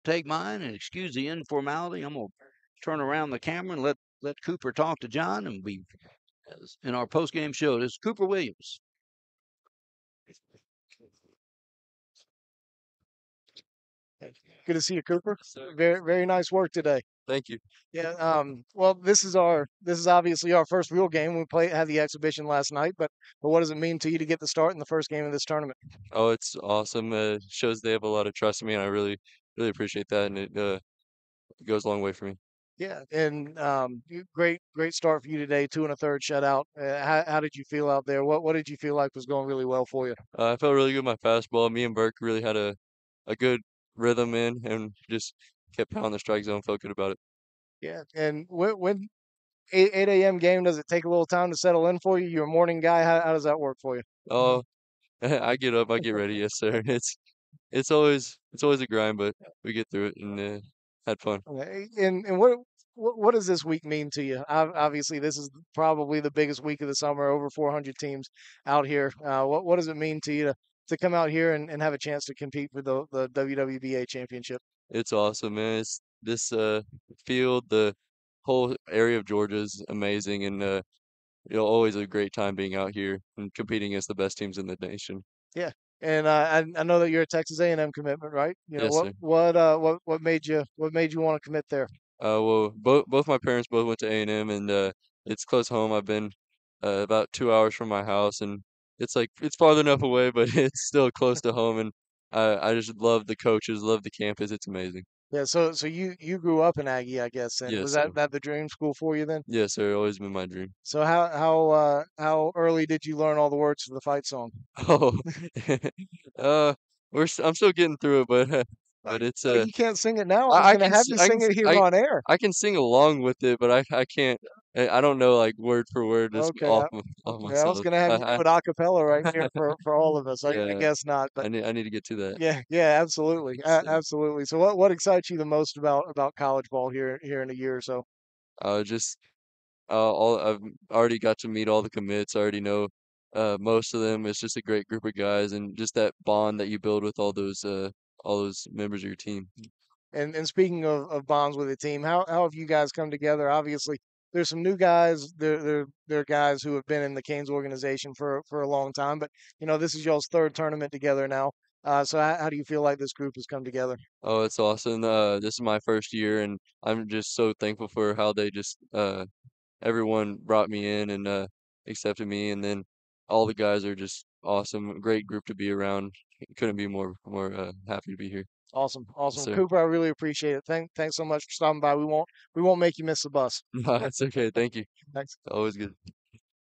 Interview with Texas A&M commitment Cooper Williams
Texas A&M Aggies commitment Cooper Williams sat down for a post-game interview at the WWBA Championships in Marietta, Ga. on July 16, 2023.